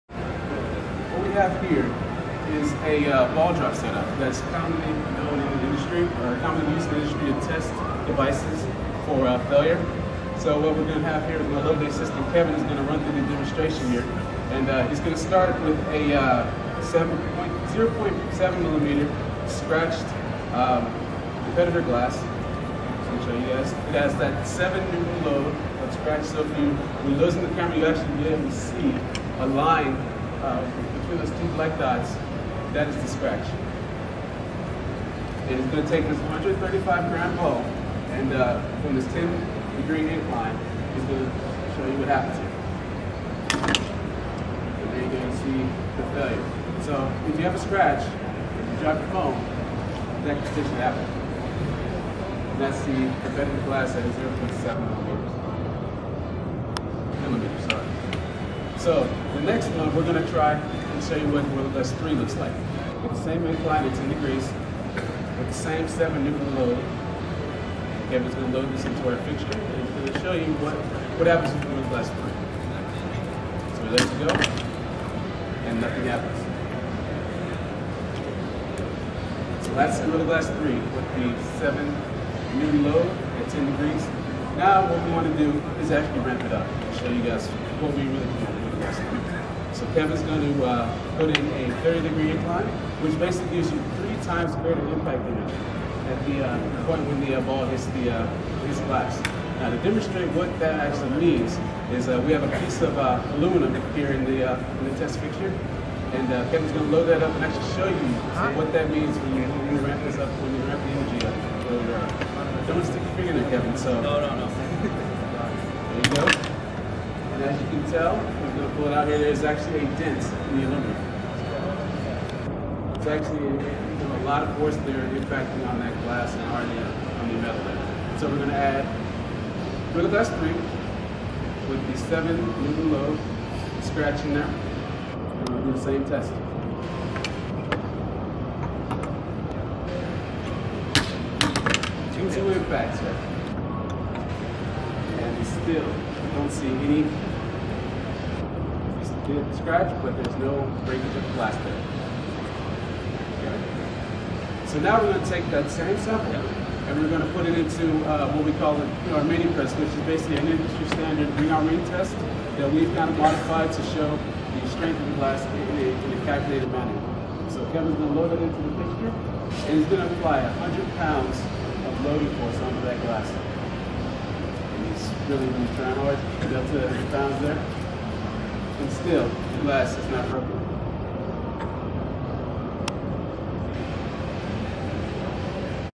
Corning Gorilla Glass 3 Demo at CES 2013
At CES 2013, Corning demonstrates the strength of the new Gorilla Glass 3.